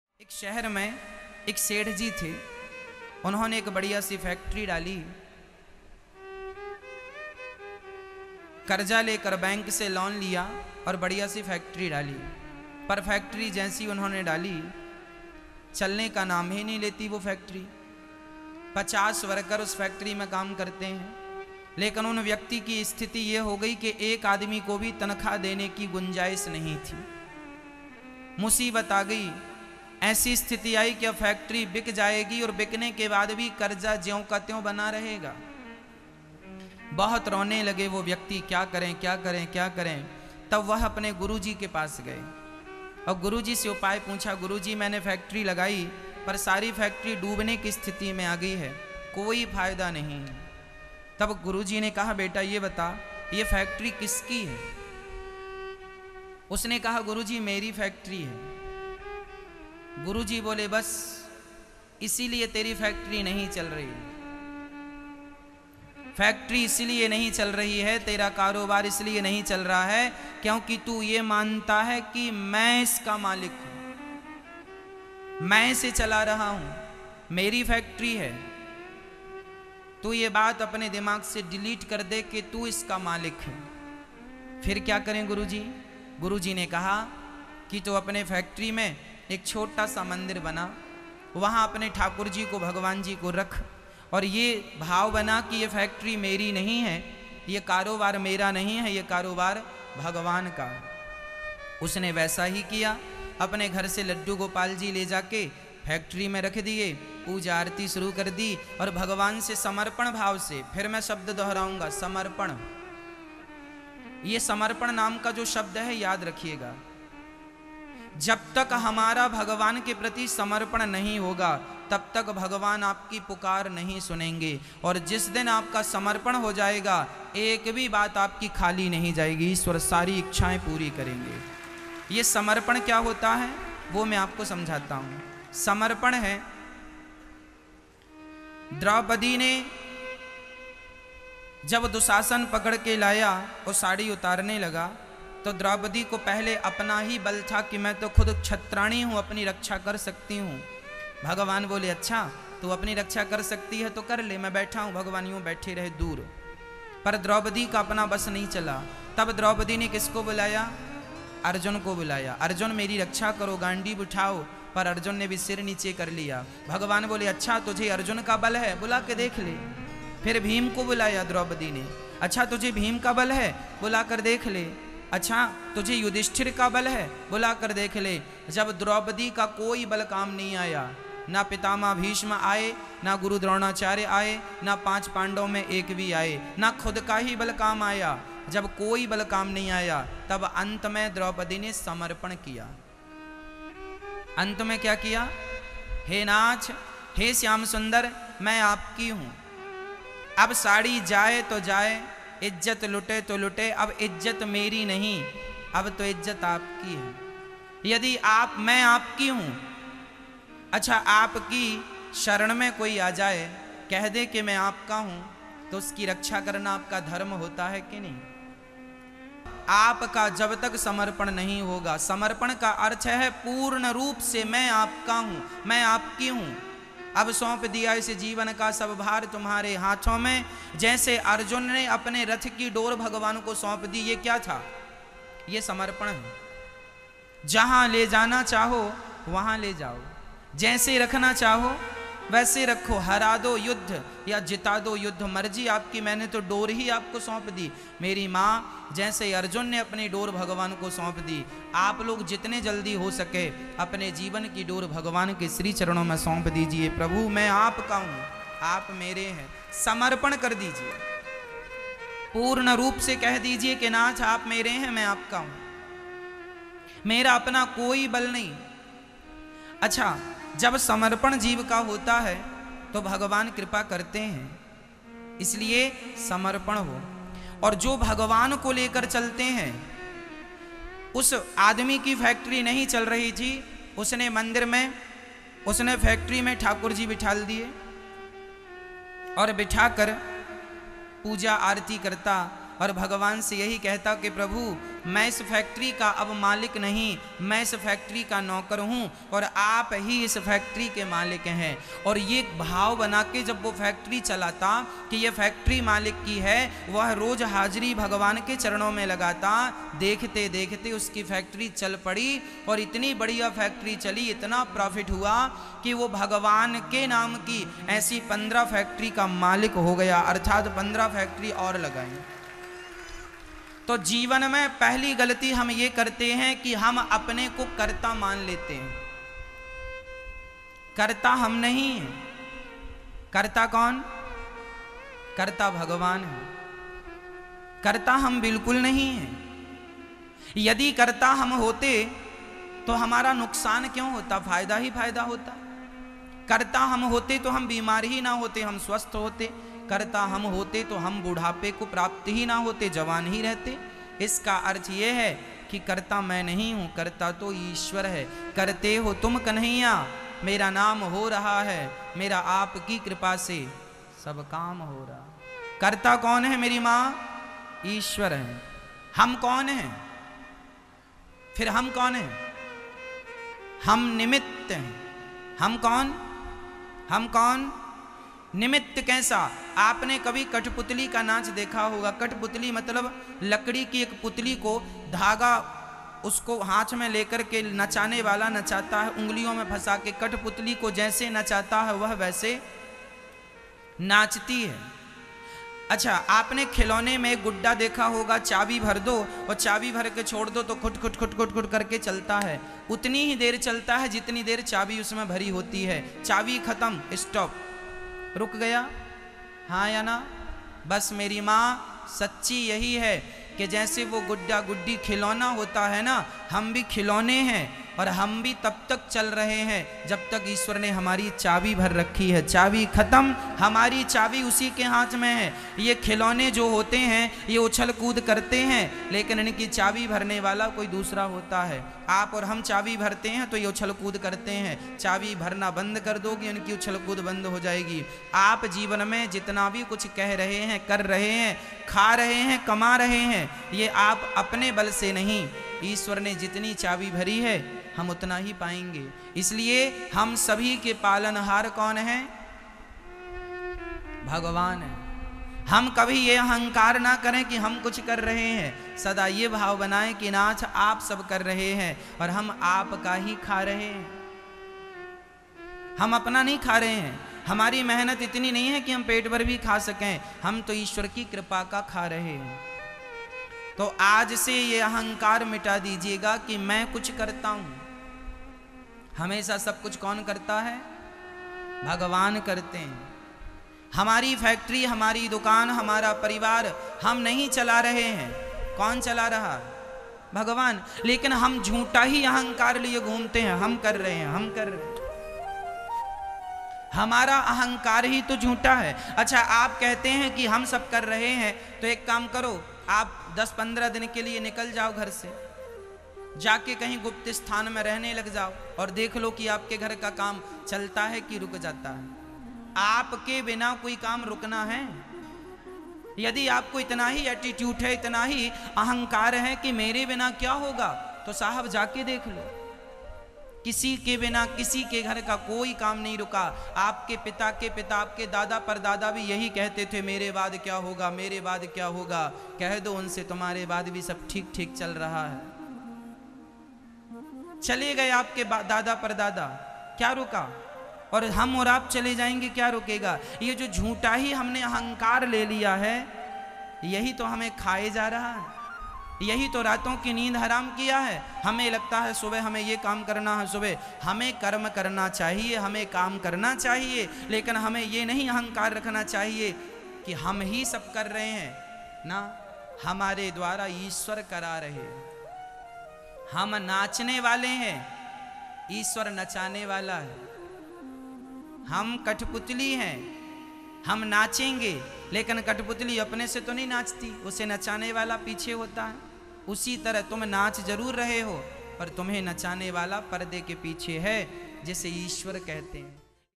व्यापार में जा रहा है घाटा तो करें ये उपाय । रातों रात चमक जाएगी आपकी किस्मत ।श्री अनिरुद्धाचार्य जी
FOR MORE QUERIES:
▬▬▬▬ஜ۩۞۩ஜ▬▬▬▬